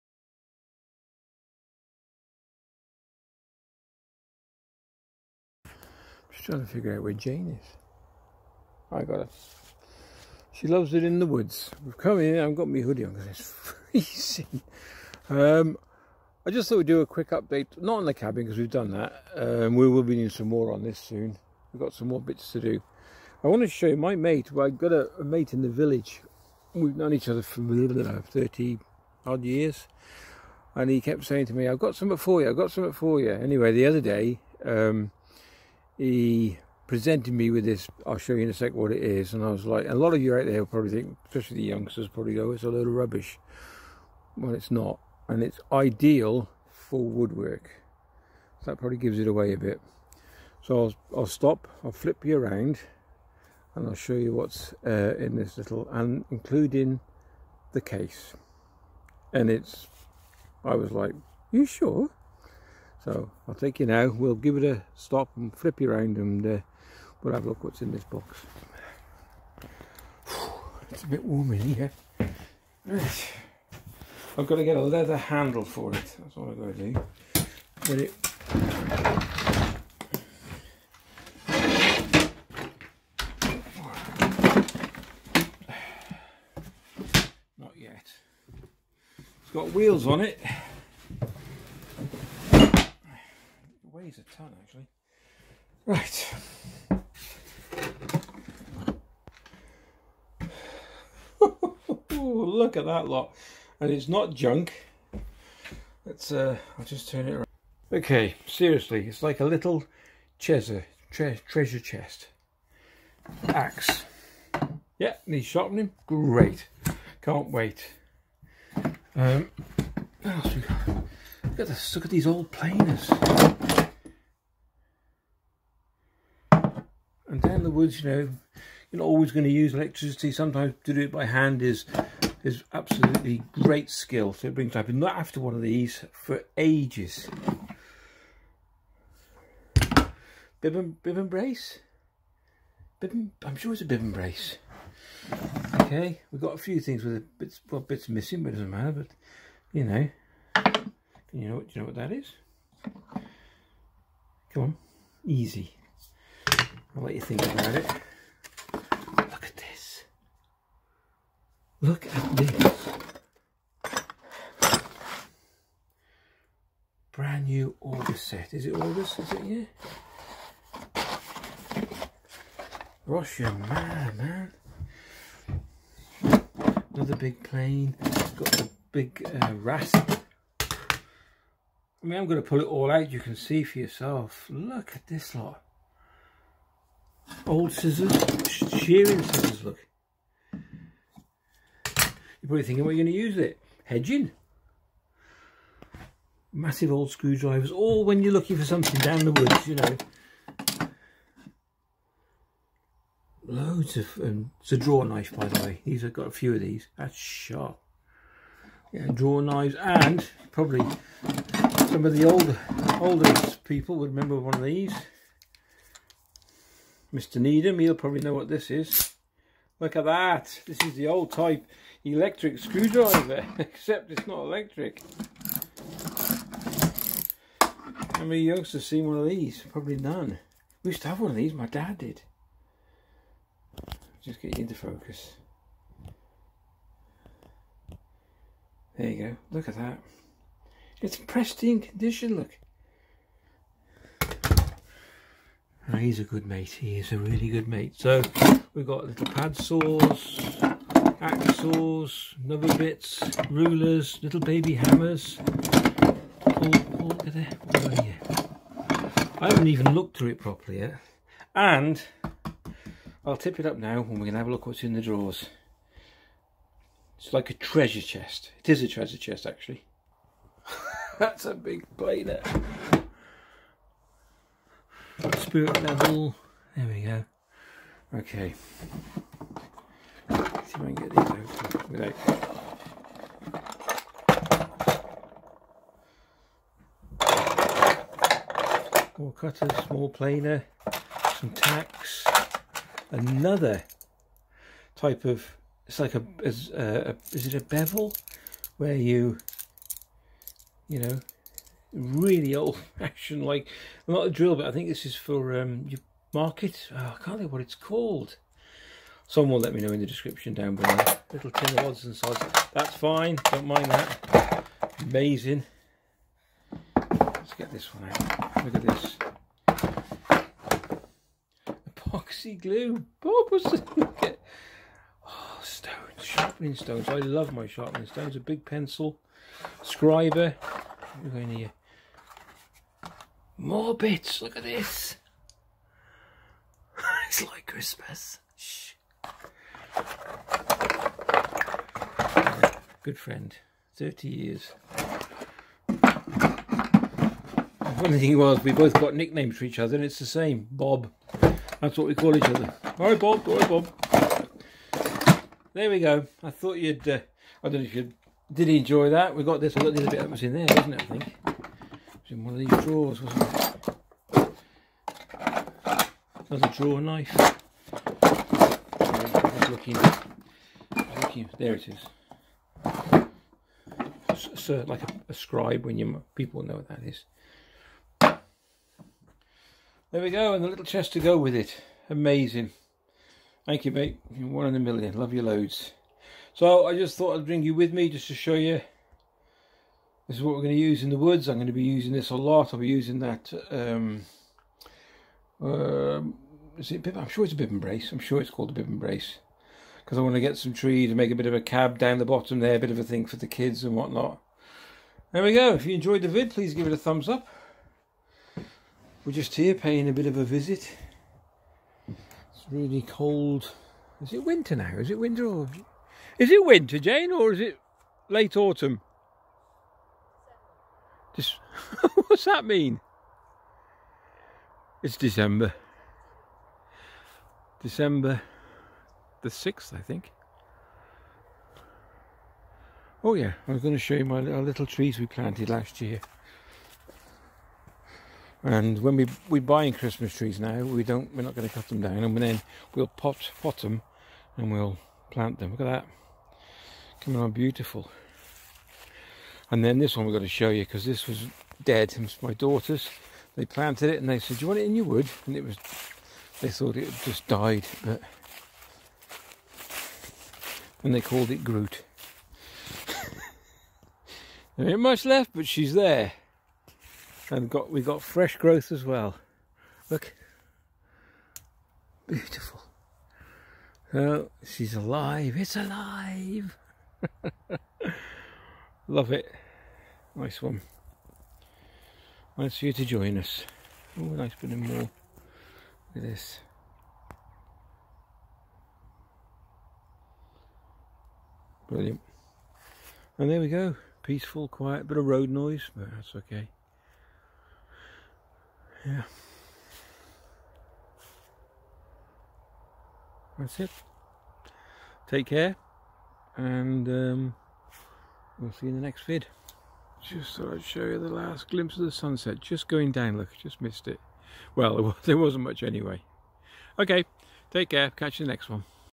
CABIN UPDATE OLD TOOL CHEST
A friend of mine for the past 30 + years  gifted me a tool box
and it is brilliant will be put to good use